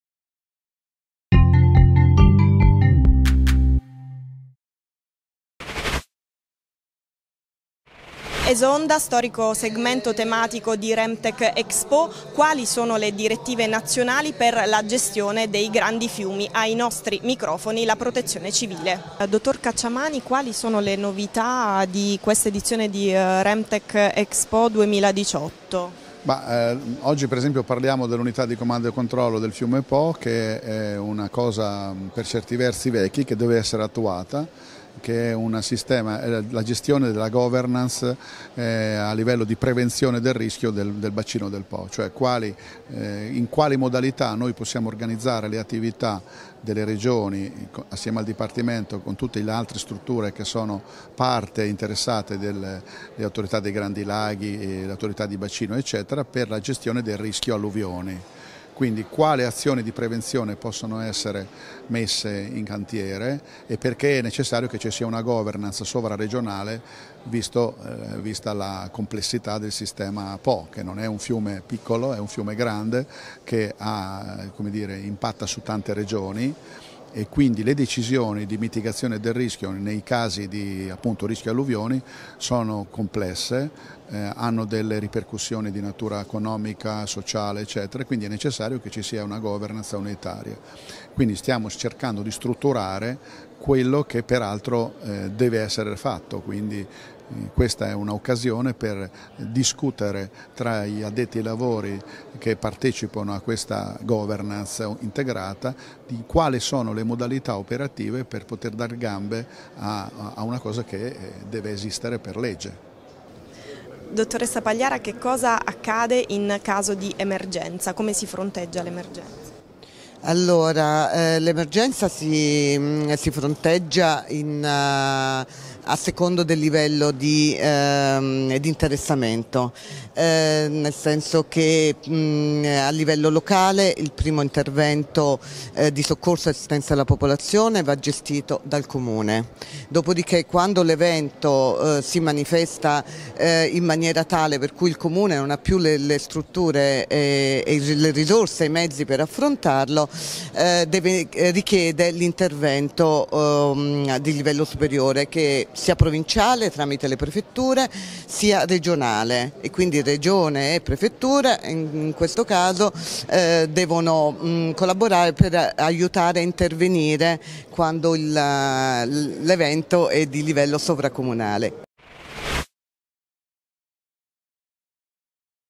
REMTECH 2018 - BACINI FLUVIALI, PREVENIRE E GESTIRE L'EMERGENZA
RemTech Expo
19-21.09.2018
Ferrara Fiere
Come governare al meglio i bacini fluviali per prevenire e gestire il rischio di alluvioni? Se n'è discusso a RemTech Expo nell'ambito del segmento tematico Esonda. Oggi focus sulla governance del fiume Po.